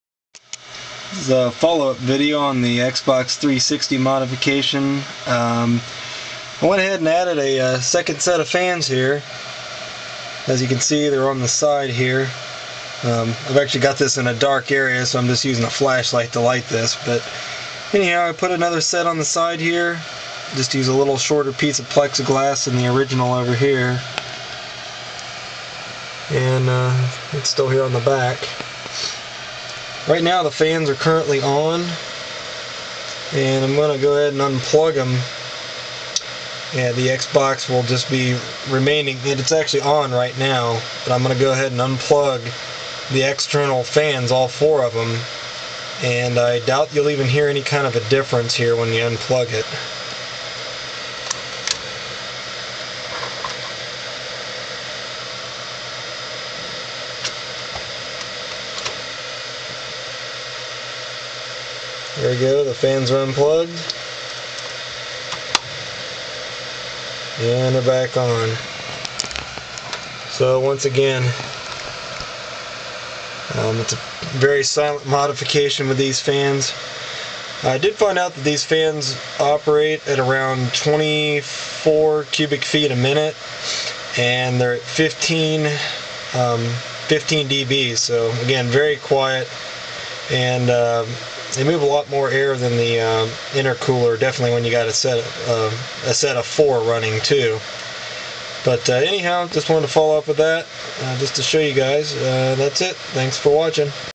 Xbox 360 external fan mod that WON'T void your warranty 2
I added a second set of fans to the right side of the 360.  These fans are forcing air INTO the 360 and the ones on the back are supplimenting the exhaust.